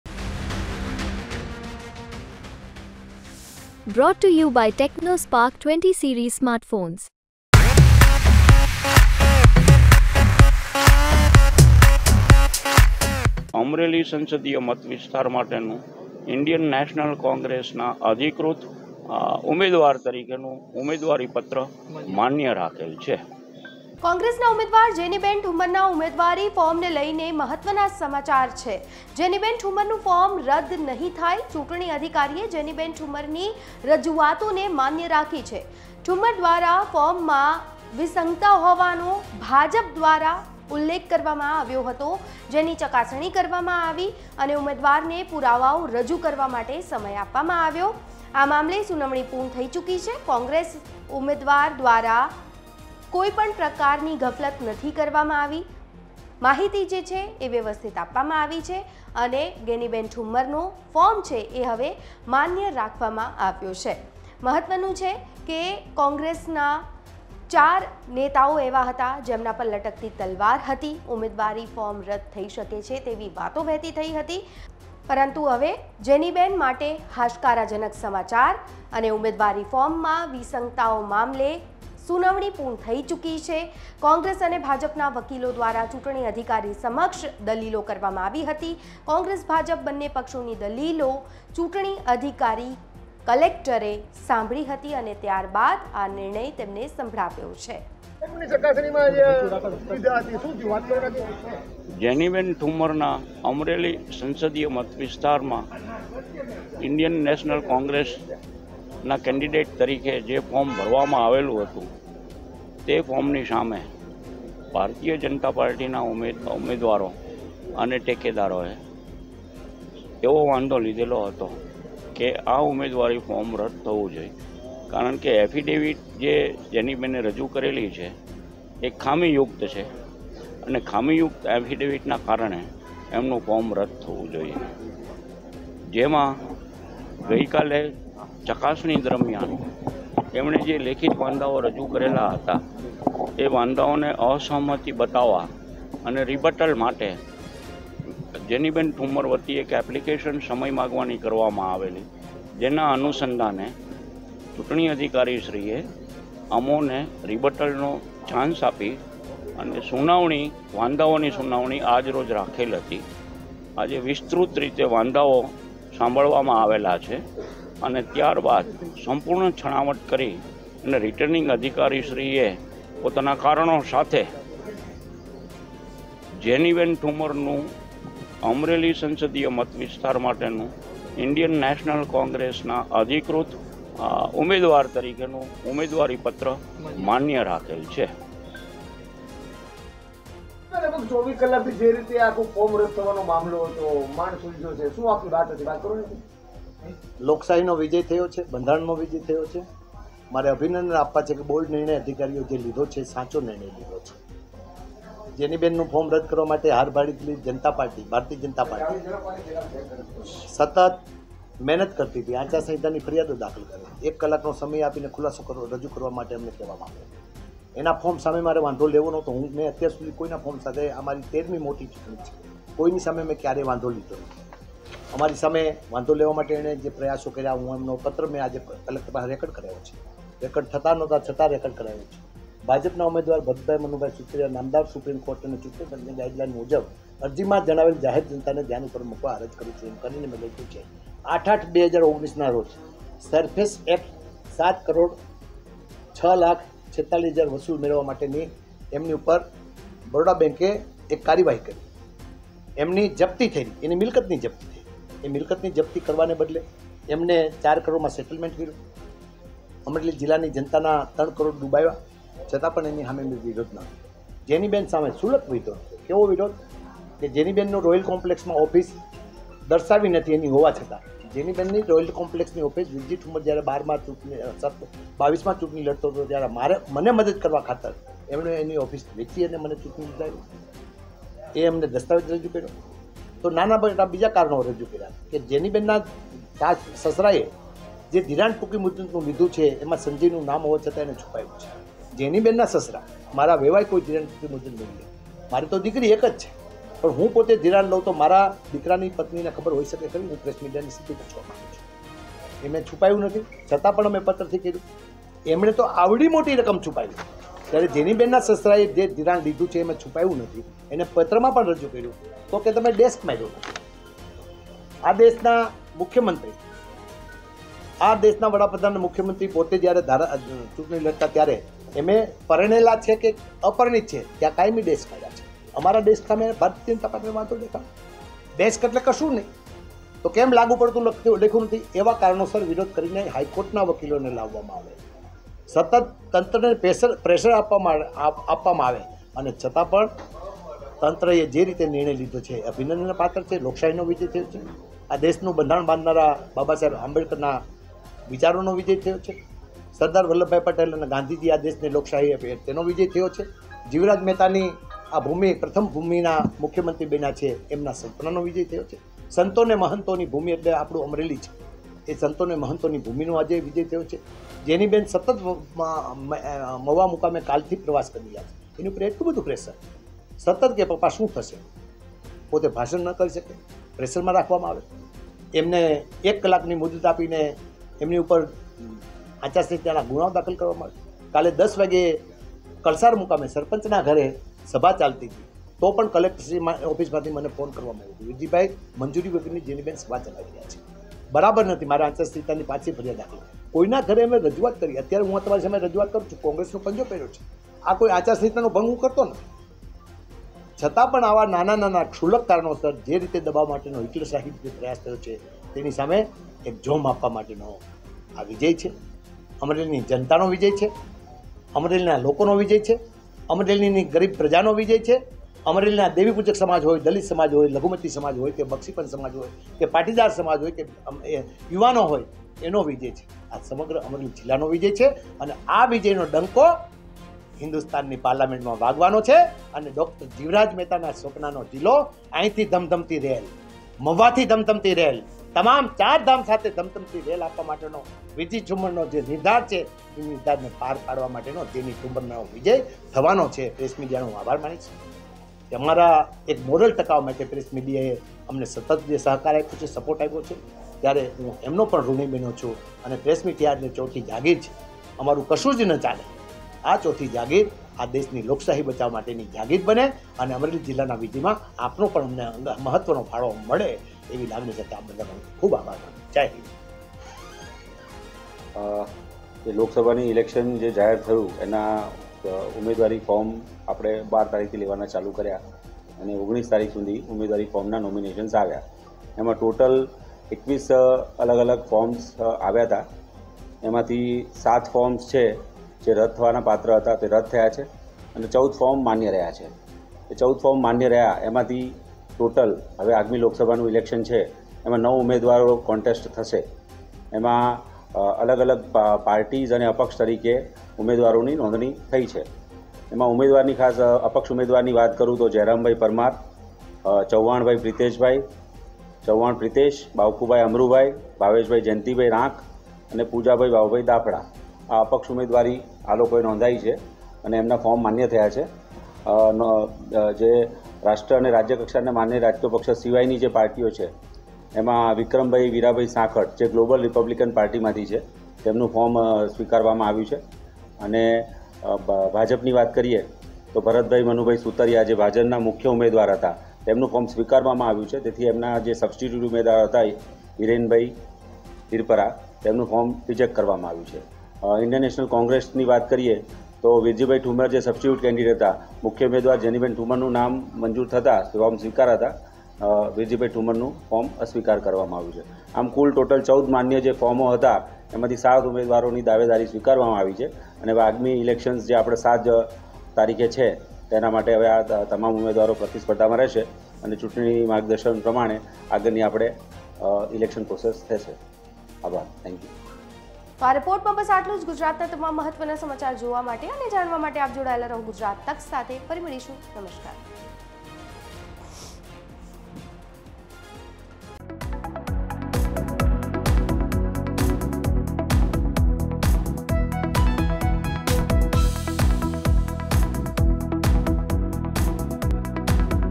Congress માં હરખની હેલી, Jenniben Thumar નું ઉમેદવારી ફોર્મ મંજૂર!| Gujarat Tak
કોંગ્રેસનાં ઉમેદવાર જેનીબેન ઠુમ્મરનાં ઉમેદવારી ફોર્મને લઈ મહત્વનાં સમાચાર મળી રહ્યા છે. જેનીબેન ઠુમ્મરનું ફોર્મ રદ્દ નહી થાય. ચૂંટણી અધિકારીએ જેનીબેન ઠુમ્મરની રજૂઆતોને માન્ય રાખી છે. જેનીબેન ઠુમ્મર દ્વારા ફોર્મમાં વિસંગતતાઓ હોવાનો ભાજપે આક્ષેપ કર્યો હતો
Description Links